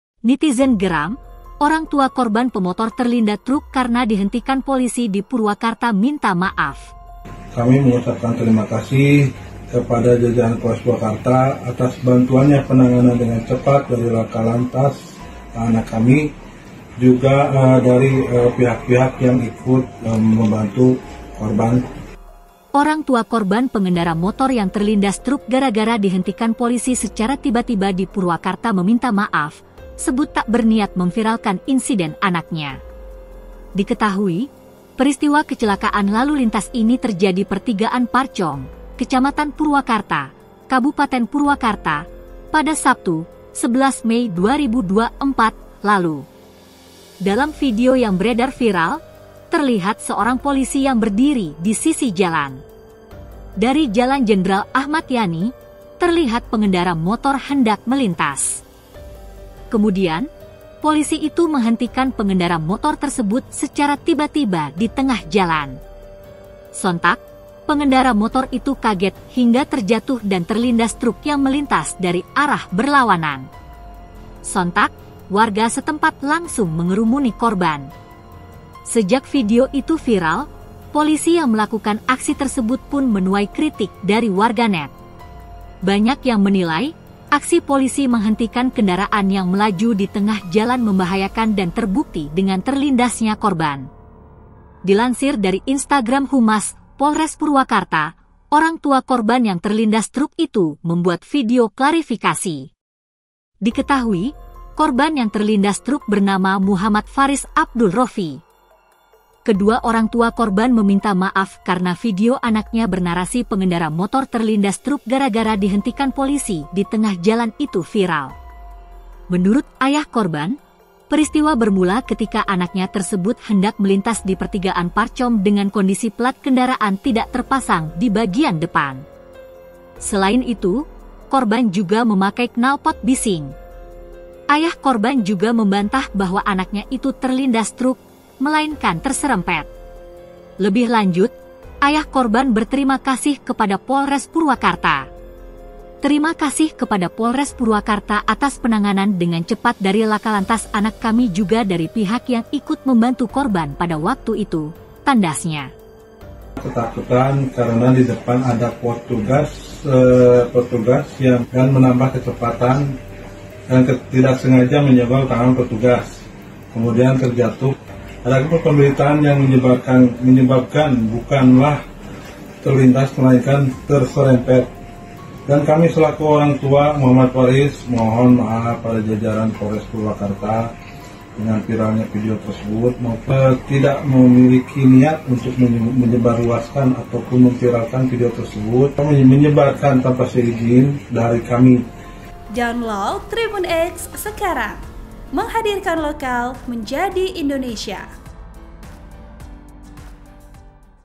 Nitizen Geram, Orangtua Korban Pemotor Terlindas Truk Saat Dihentikan Polisi Minta Maaf
Orang tua korban pengendara motor yang terlindas truk gara-gara dihentikan polisi secara tiba-tiba di Purwakarta meminta maaf, sebut tak berniat memviralkan insiden anaknya.

Tribun Sumsel/ Ebit